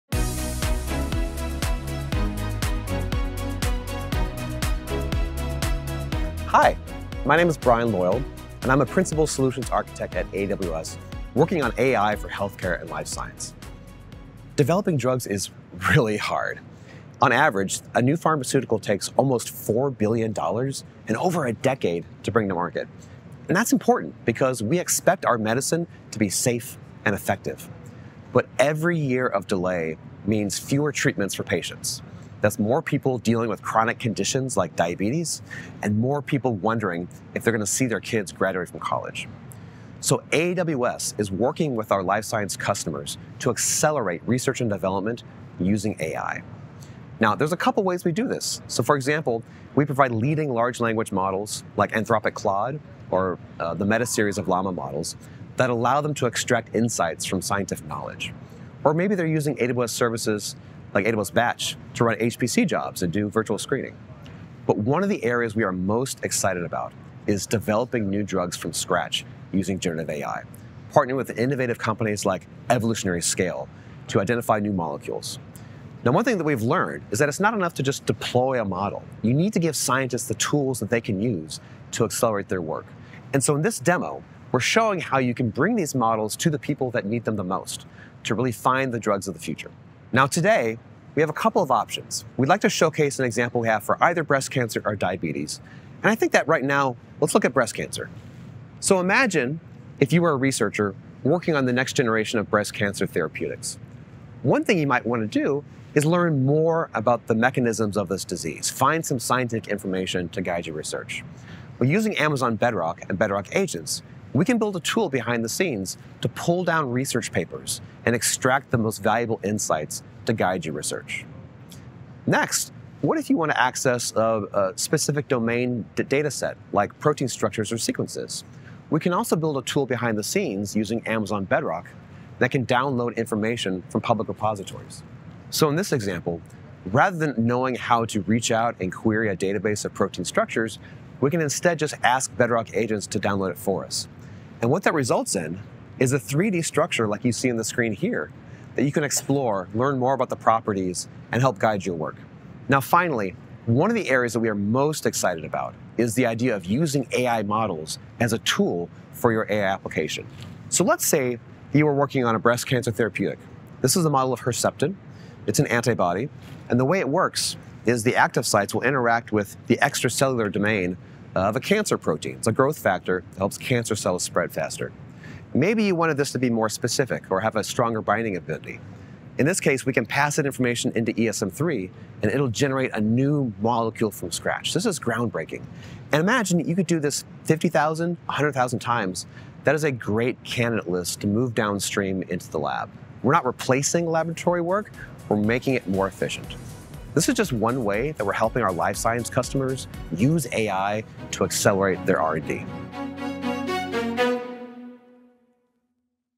Demo: Transforming drug discovery with AI on AWS | Amazon Web Services
This immersive demo shows how generative AI on AWS - powered by Amazon Bedrock Agents, and bioFMs like ESM3 available on AWS SageMaker and AWS HealthOmics - can streamline traditional R&D processes into efficient, AI-powered discovery workflows, through an intuitive, no-code interface.

Why AWS?